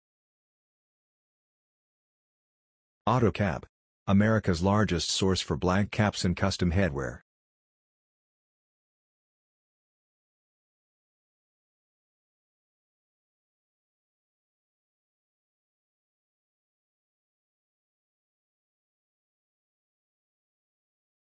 OTTO CAP - Style No. 39-090 5 Panel Mid Profile Mesh Back Trucker Hat
Style No. 39-090
5 Panel Mid Profile Mesh Back Trucker Hat
Denim
Structured Firm Front Panel
Front: 100% Cotton
Back: 100% Polyester